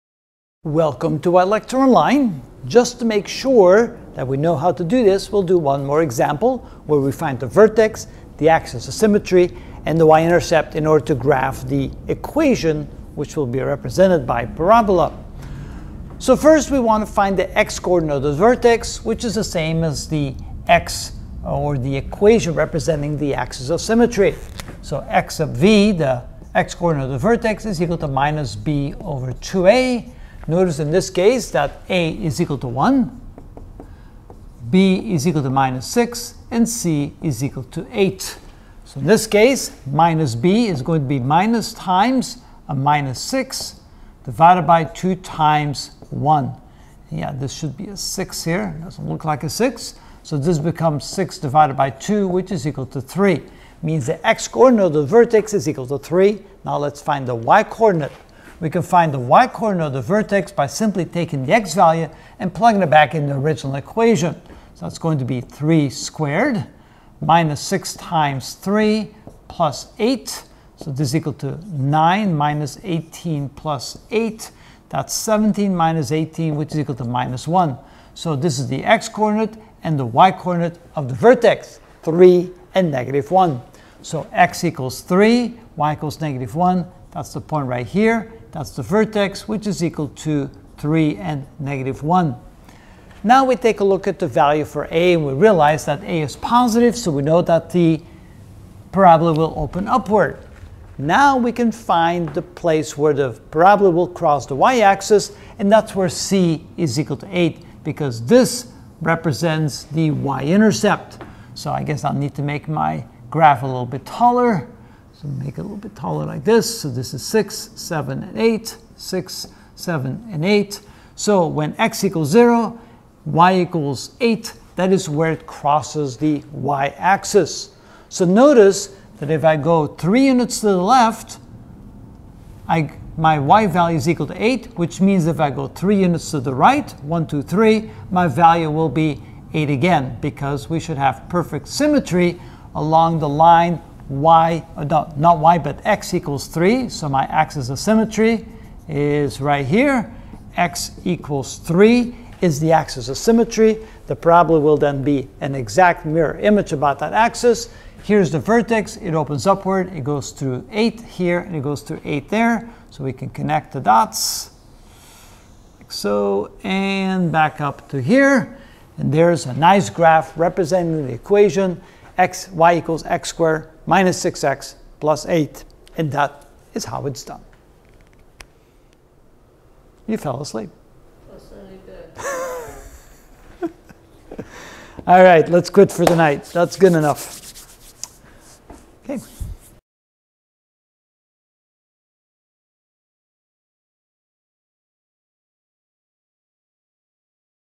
Algebra - Ch. 37: Parabola (10 of 22) Find the Vertex and y-Intercept Example 2
We will graph the (parabola) general equation y=x^2-6x+8 by finding the vertex, axis of symmetry x=(-b)/(2a), and the y-intercept=c. Example 2